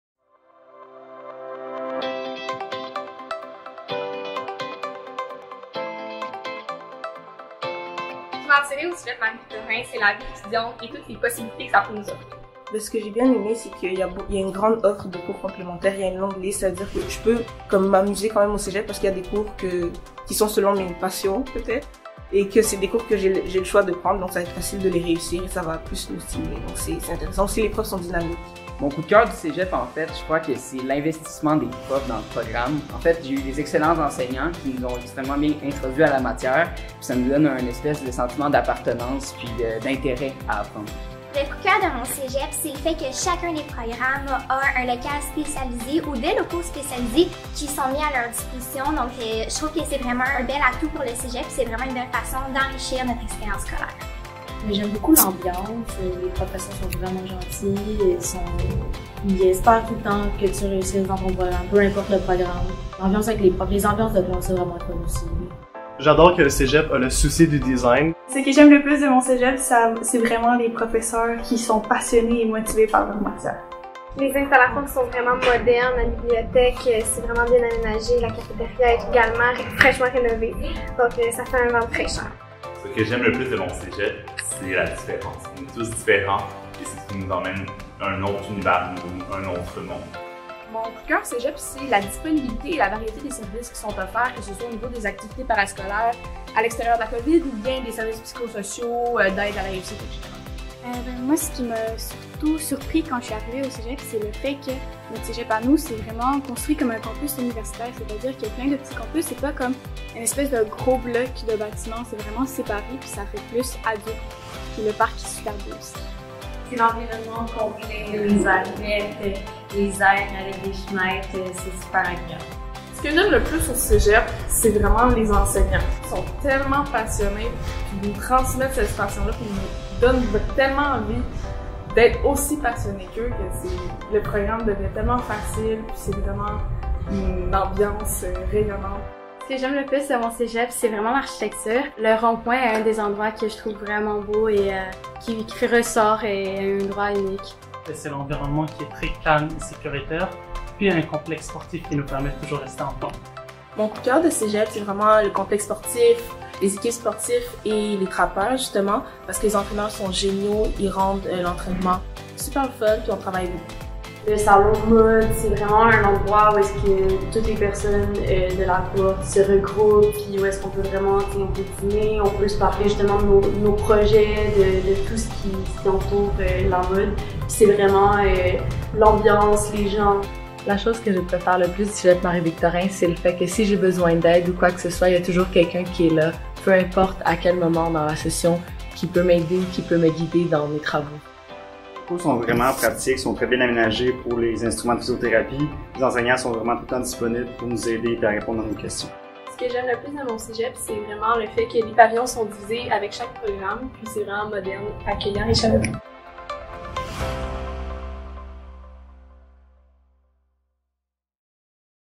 La Brigade 2021 révèle ses coups de cœur du Cégep Marie-Victorin
Découvrez les coups de cœur des membres de la brigade 2021 du Cégep Marie-Victorin!

Par la mise en place de cette brigade, le Cégep Marie-Victorin souhaite permettre aux futures étudiantes et futurs étudiants de se projeter au collégial dans l'un de nos nombreux programmes!  Lisez et visionnez leurs témoignages inspirants